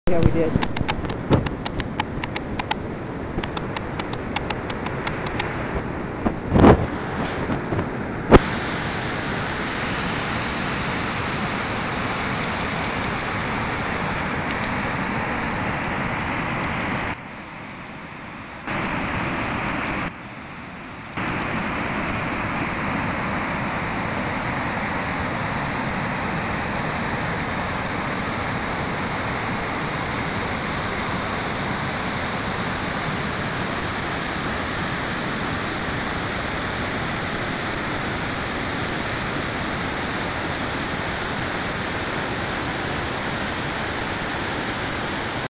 UPPER DEADMAN CREEK THAT LEADS TO WATERFALL
UPPER PORTION OF DEAD MAN CREEK WATER FALL IN MAMMOTH.  THIS WAS TAKEN ON 6/26/2010 AS YOU CAN SEE THE CREEK IS QUITE FULL.